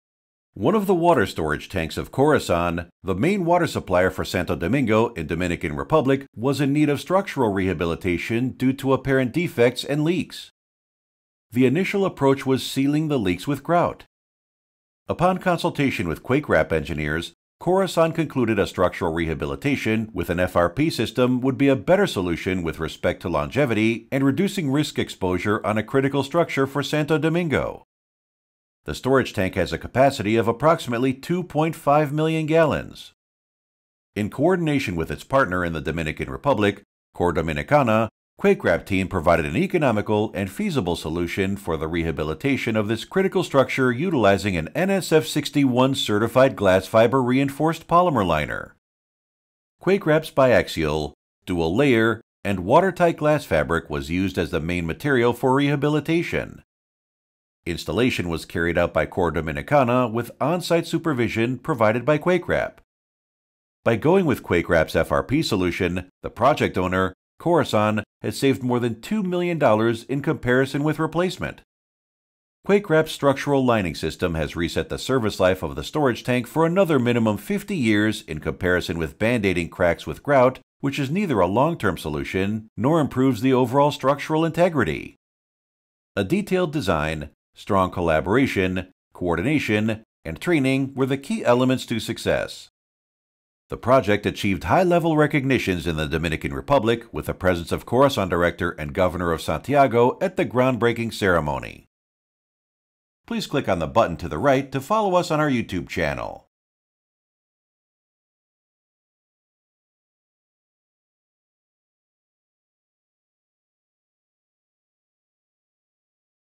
Repair of Leaking Concrete Water Storage Tank with FRP
A leaking reinforced concrete water storage tank in Santa Domingo, Dominican Republic was repaired. The tank had multiple cracks and leaks.  Layers of resin-saturated QuakeWrap glass fabric that are NSF-61 certified for potable water were used to internally repair the tank. The repair not only stops leaks from the tank, it also strengthens the walls of the tank. The repair was completed on schedule and will provide decades of maintenance free service life for the tank.  Support of our local associates Core Dominicana in this project is gratefully acknowledged.